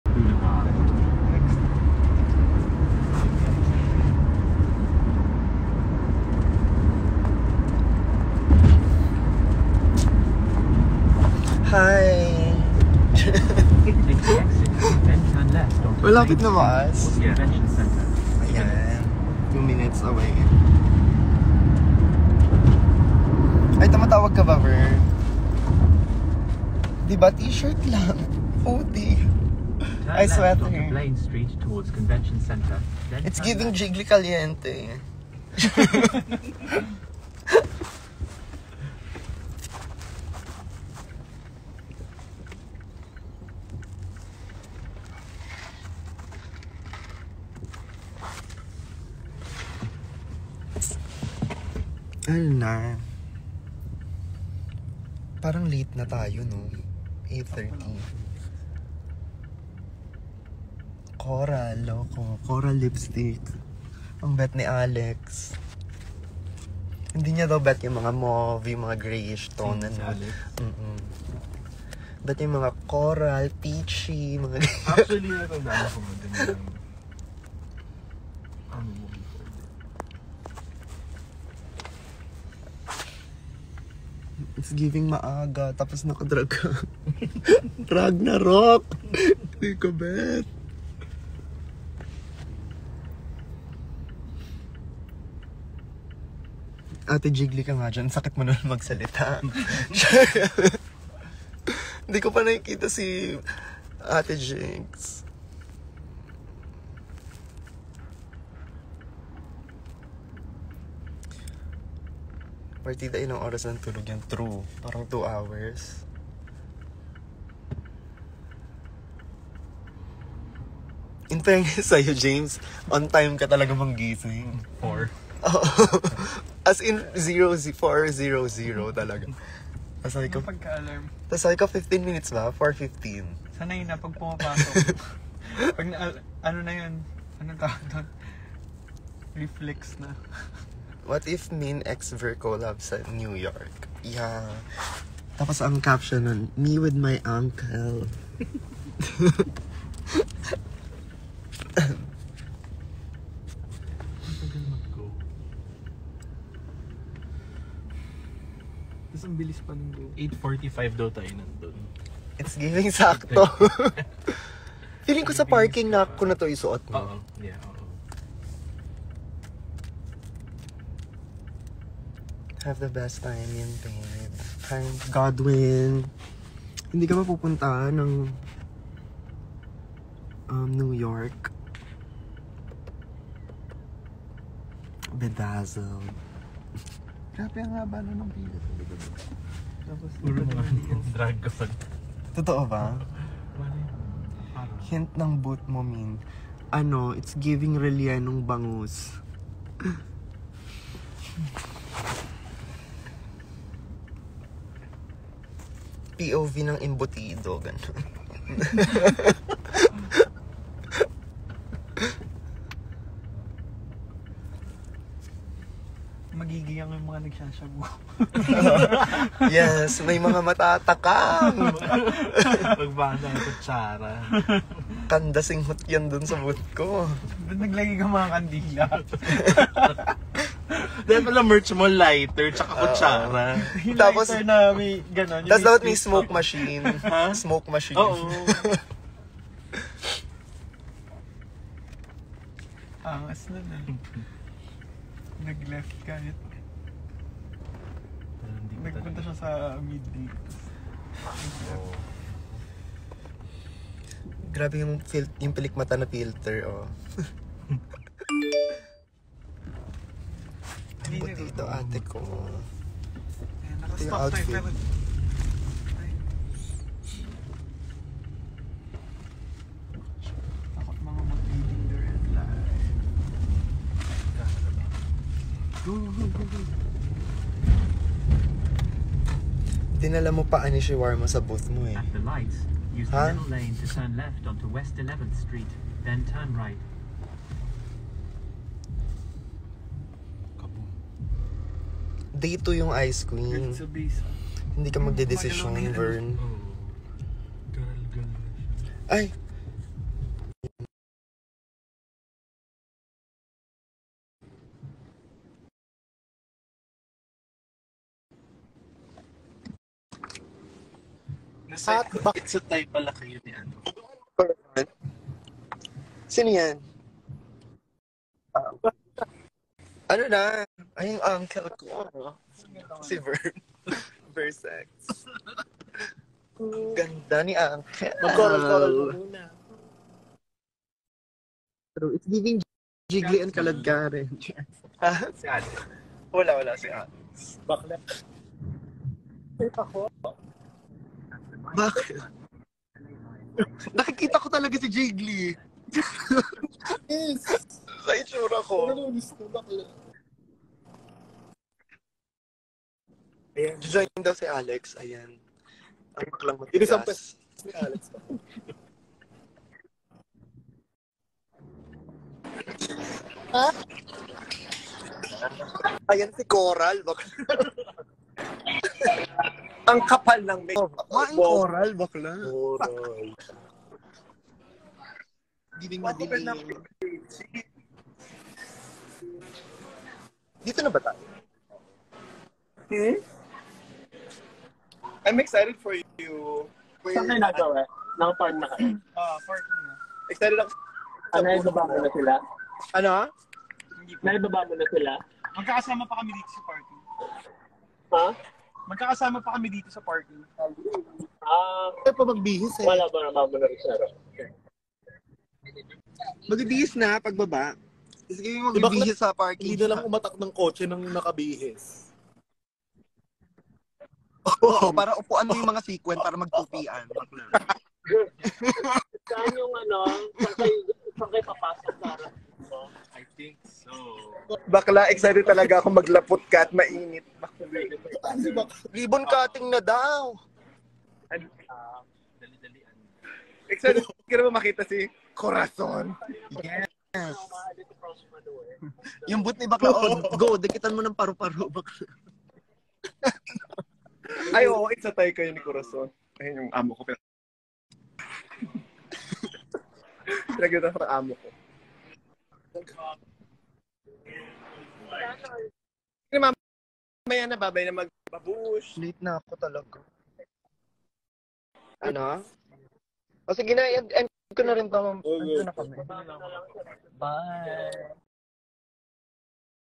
Minty Fresh - Instagram Live - 12 May 2023 Part 2
Drag Race Philippines Season 1 Minty Fresh with versex and divasoria - Instagram live - Friday, May 12, 2023 Part 2